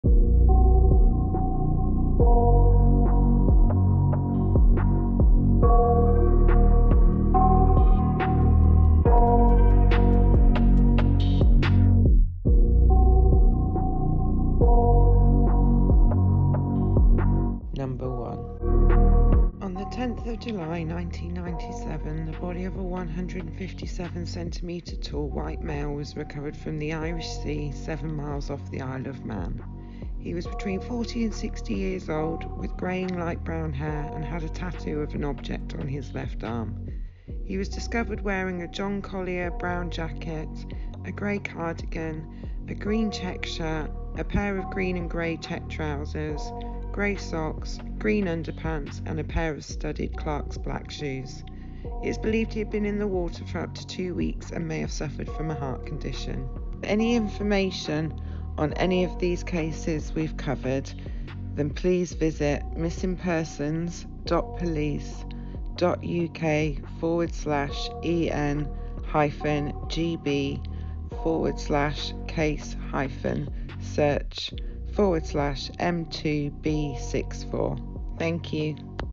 Unsolved Missing Person Cases - Isle of Man Police
Here are some of the missing persons cases which are still unsolved within the Isle of Man Police.

If you have any information on the cases covered, please